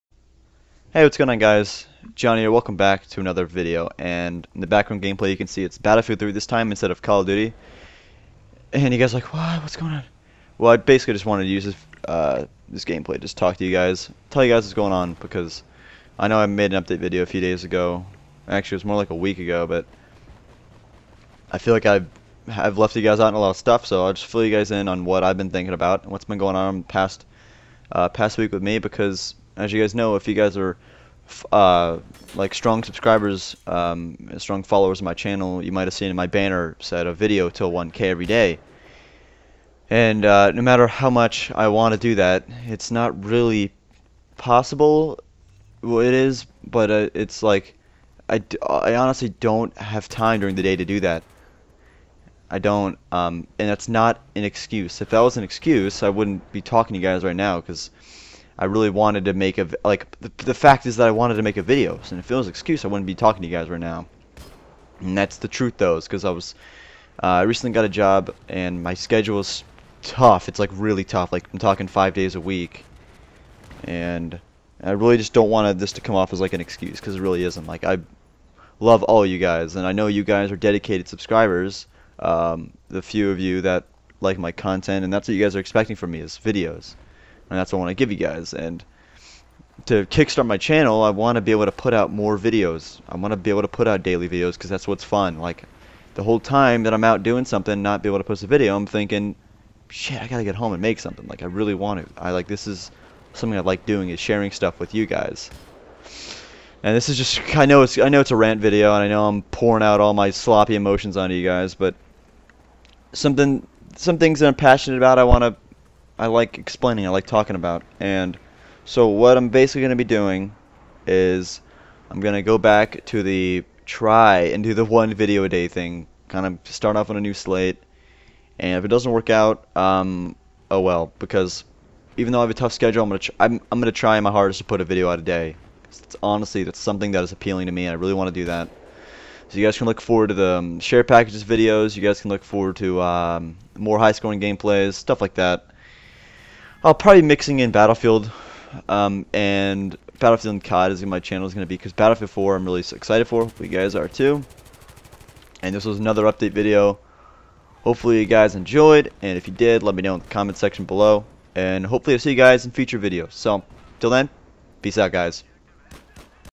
Another One Of Those Ranting Videos..
The color correction, in my opinion, looks kinda fucking cool.

READ:
Some other things I forgot to mention. Black Ops 2 is kind of burning me out. I get frustrated soo much at the game now, it's hard for me to play it and have fun. I will still post COD videos, I just want to post more gameplay that I enjoy, such as BF3 for instance. Hopefully you guys can understand and be supportive. Much love

- John